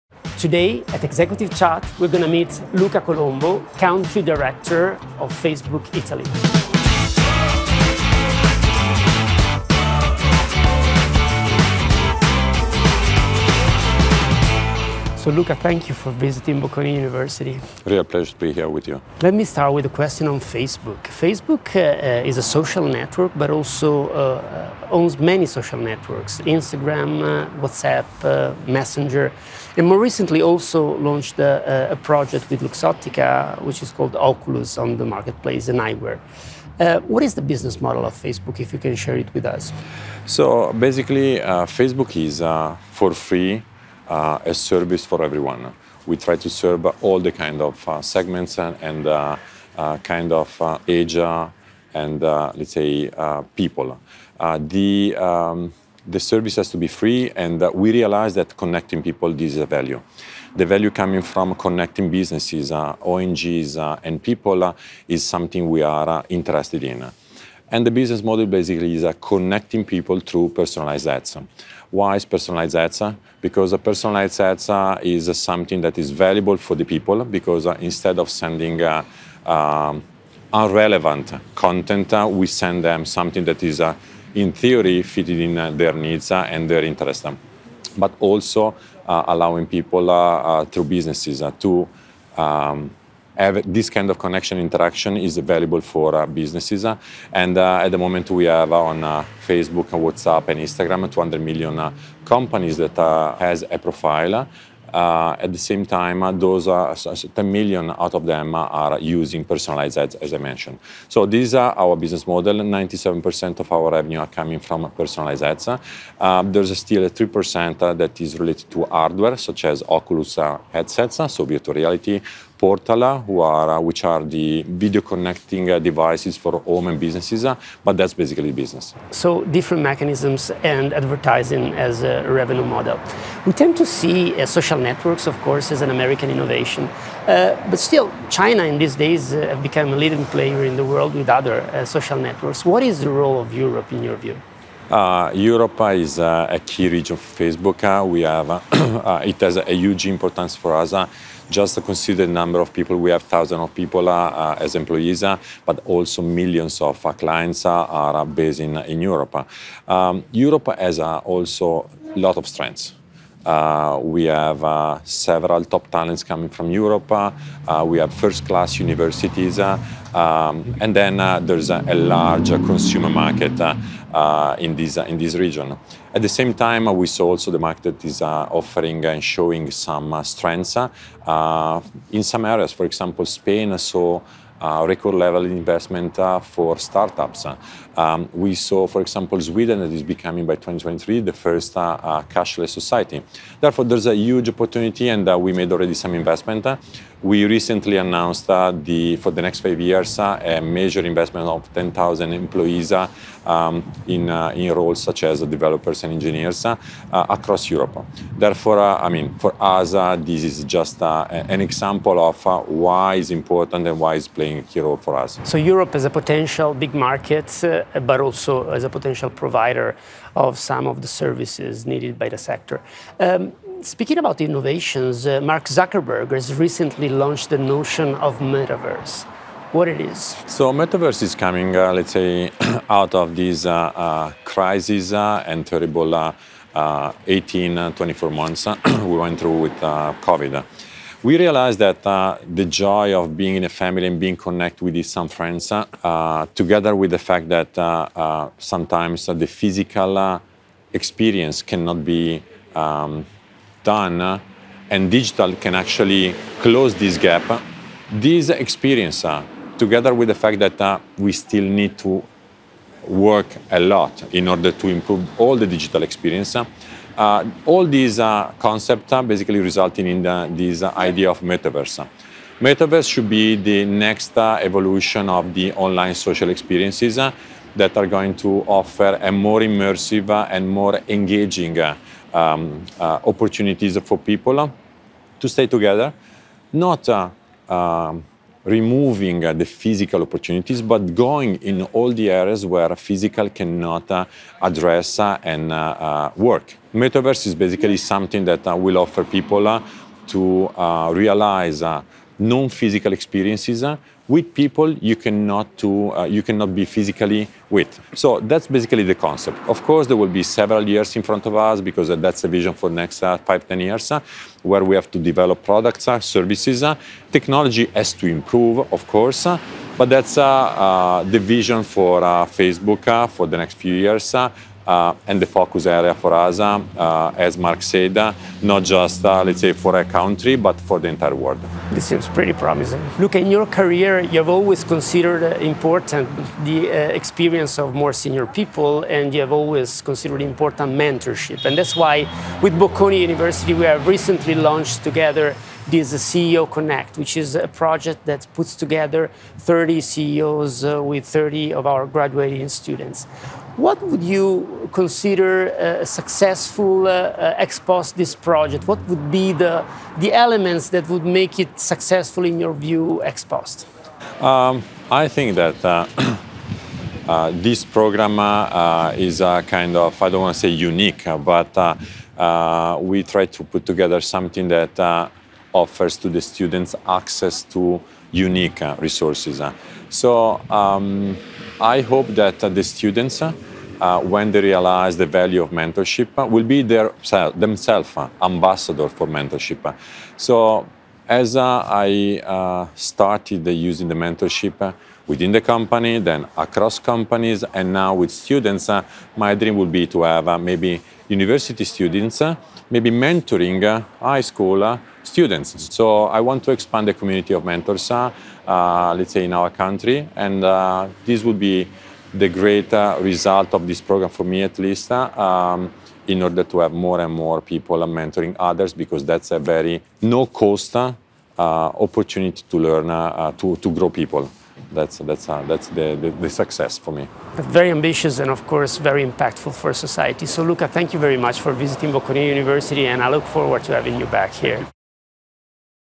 Luca Colombo, Country Director Italy, Facebook | Podcast #4
“Connecting people is a value”, highlights Luca Colombo, Country Director at Facebook Italy, discussing the company’s business model in a conversation in the Executive Chats series with Rector Gianmario Verona.

Looking at the future, “the metaverse should be the next evolution of online social experiences. Experiences that should offer more immersive and engaging opportunities for people. The metaverse is our vision for the next 5-10 years – now we have to develop the necessary products and services.”

Colombo has also helped Bocconi develop the new ‘CEO Connect - Your Executive Mentor’ program which will see 30 Bocconi MSc students each paired up with 30 top CEOs who will mentor them. “The program is unique, giving students access to unique resources. I want to expand the community of mentors in Italy,” says Colombo.